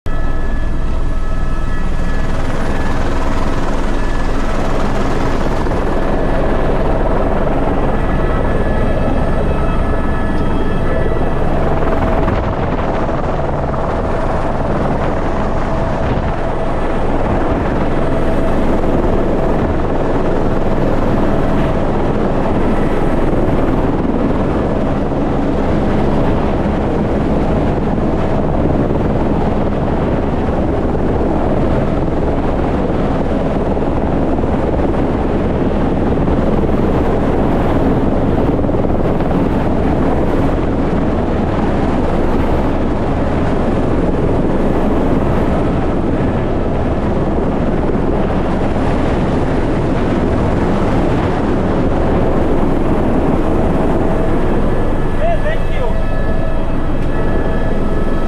DRAMATIC RESCUE: Driver in overturned truck trapped in Kenya river
A truck driver was rescued from a rising river in Kenya.

Video provided by Sheldrick Wildlife Trust shows the wildlife conservation group's helicopter slowly being lowering above the Galana River, where a man was trapped in an overturned truck for more than six hours.